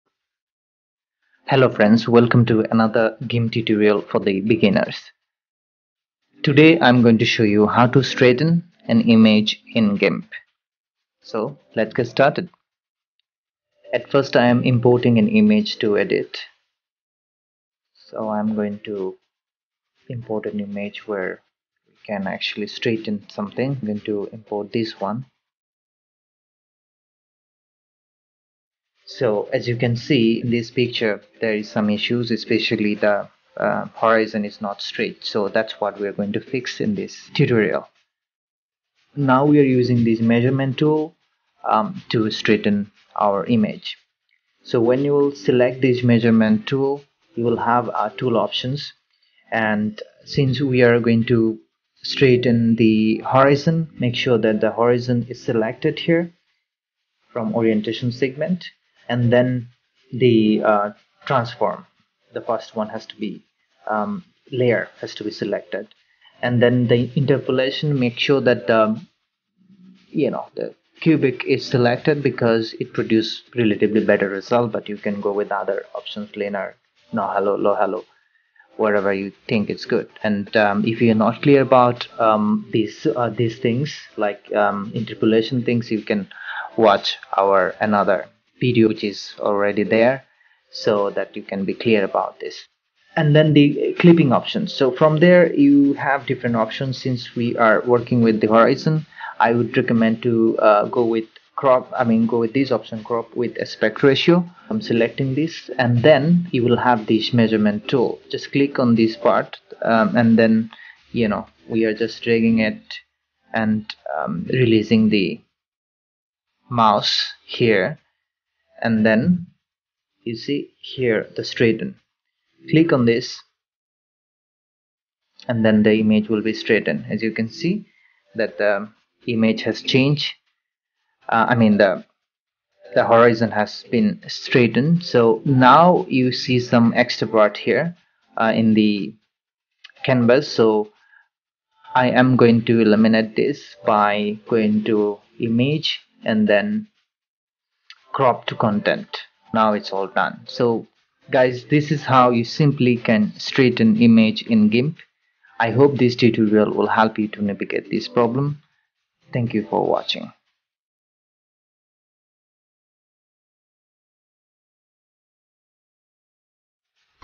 How to Straighten an Image In Gimp 2.10.20 | Gimp Tutorial for the Beginners 2020
This video will demonstrate the process of image straightening.

Thanks,
LI